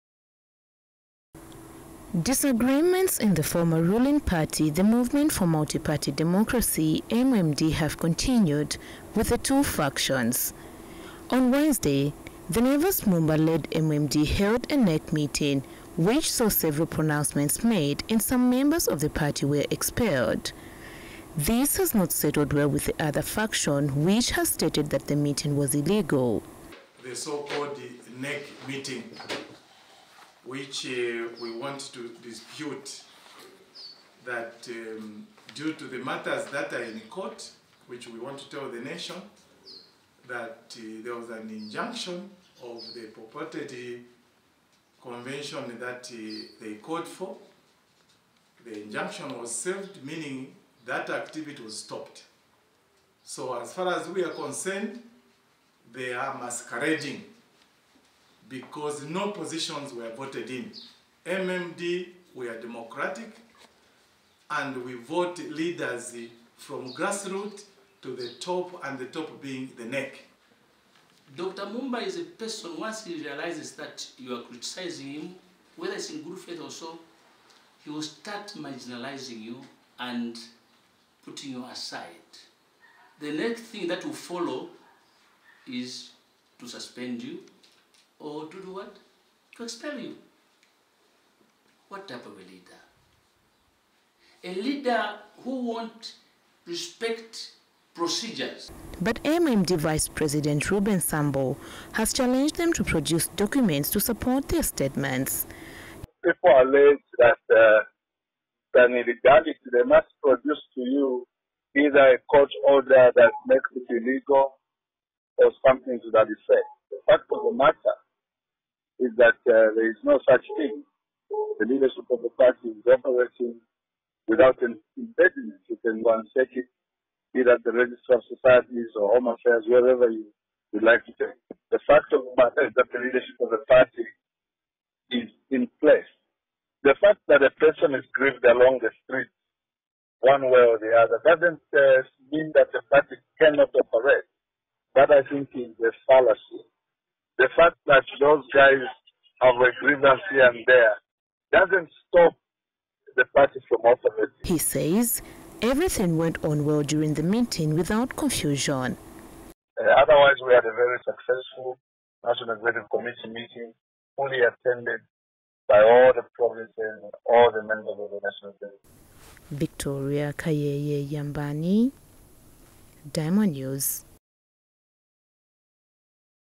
MMD IN MORE INTERNAL WRANGLING
Movement for Multi-Party Democracy (MMD) Founder Member, Guston Sichilima says the recently held National Management Meeting by Dr. Nevers Mumba is illegal because there are no new office bearers since the expiry of his mandate.

Catch the diamondnewsdesk every day at 20:00 hrs on Channel 271 DStv | GOtv 99 | TopStar 110 | Airtel TV App.